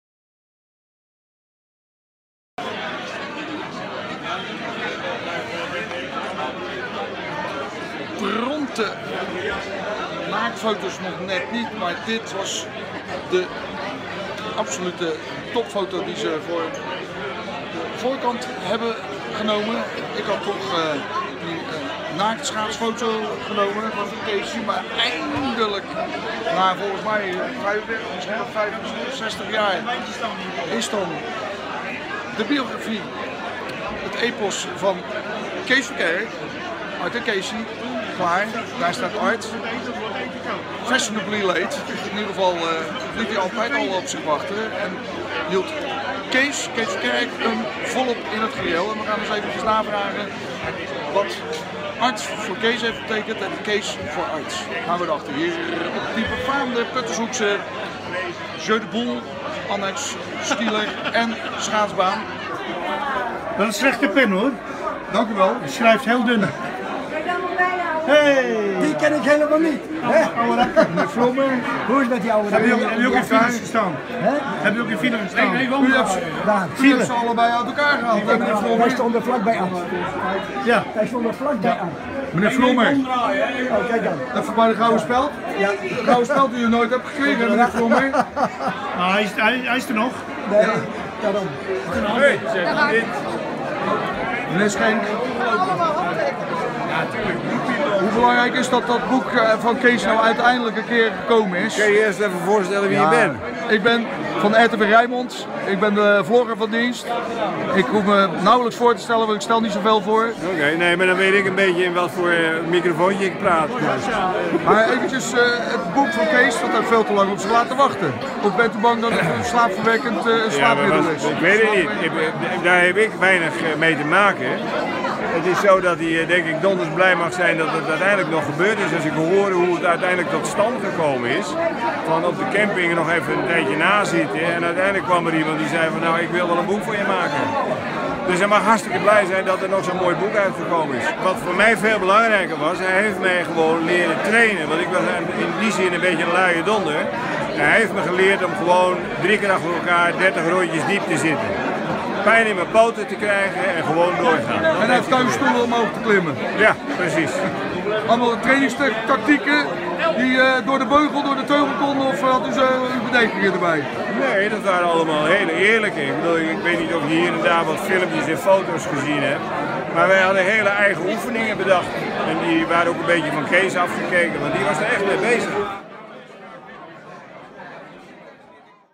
Keessie Verkerk's schaatsheldenstatus eindelijk in biografie samengevat
Puttershoekse schaatslegendaricus Kees Verkerk uit het Rotterdamse polderachterland de Hoeksche Waard heeft eindelijk z'n langverbeide biografie. Een decennialanng gekoesterde wensdroom wordt daarmee, dankzij Noord-Hollands Dagbladsportchef en oud-FC Utrecht- en Volendamvoetbalbelofte Jeroen Haarsma hiermee, bewaarheid. Angstgegner en compaan Ard Schenk nam met behartensvolle woorden van genegenheidswaarde het eerste exemplaar van het maniakale trainingsbeest gewezen kastelein, campinghouder in Noorwegen en uiterst aimabele trompettist, orgelspeler en begenadigd entertrainert in ontvangst. Op zijn eigenste Kees Verkerkijsbaan staptende ruim honderd aanwezige Verkerkfans met alle graagtes van gernten aan boord van de zich razenddazend voortbewegende memorylane-rollercoaster van Good Old Keessie, om zich vervolgens volledig onder te dompelen in het met beladen annekdotische emoties volgestorte Ard-, Keessie en ook een beetje Jan Bolsbad.
Omgeven door iconen als Sjoukje Dijkstra, Leen Pfrommer en wie al dies meer ook weer zag Jack F Kerklaan met zichtbaar genoegen de Ard- en Keessieheldenverering van de eerste massale oranjegekte-veroorzakers van weleer